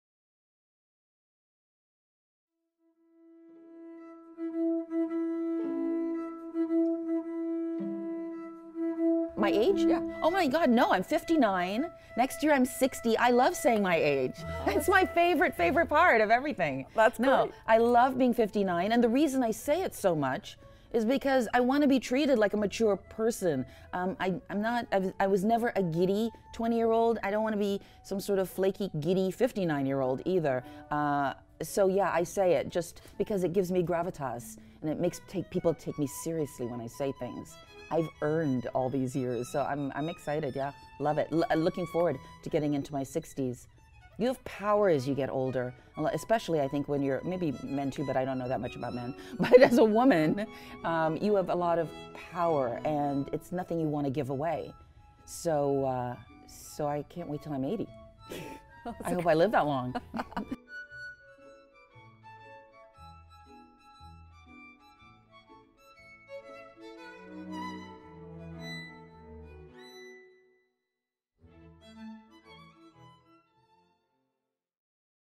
I Love My Age - A Conversation with Tonya Williams
Beloved Canadian actor and producer, Tonya Williams shouts from the rooftop, that she just LOVES saying her age.